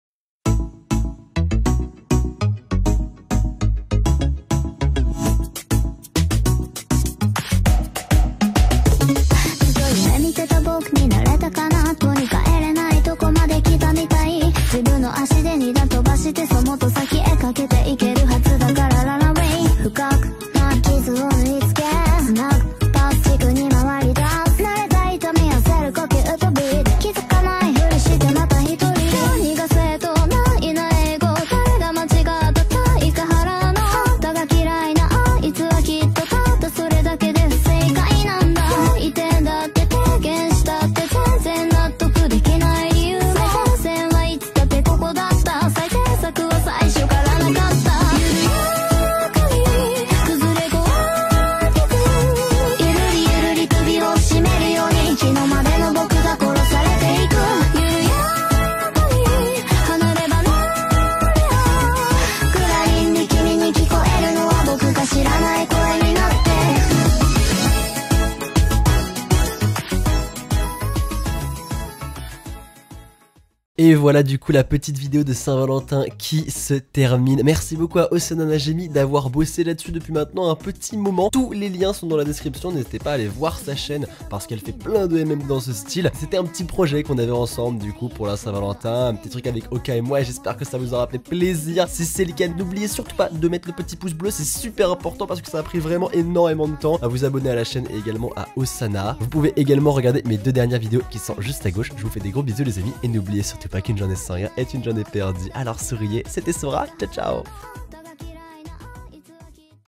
LA SAINT VALENTIN DE OKA ET SORA ! ❤ ( CLIP MMD )
Un clip de musique en mmd ? Oka ? La saint valentin ? :P
Clip mmd oka sora saint valentin français ( fr )

les crédits des effets reviennent à leurs créateurs respectifs.

★ Mon Snapchat : universdesora